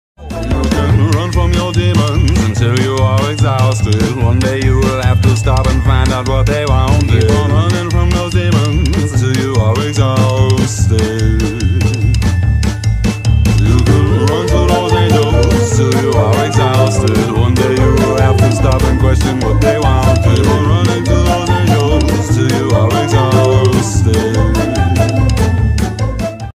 You can run from your demons Meme // Good Omens // My Au
imma be honst, i have not been working on a single video these past 2 months, as i haven’t had the motivation, and i couldn't think of any video related thing that i could possibly do with any of the GO, UTAU or any other fandoms that i am currently in

so again, sorry for that

《《 Charater(s) 》》

Aziraphale

《《 Apps used 》》

Gacha (Plus)
Capcut
Powerful Director

《《 Credits 》》

Good omens belongs to Terry Pratchett and Neil Gaiman

《《 ships 》》

None))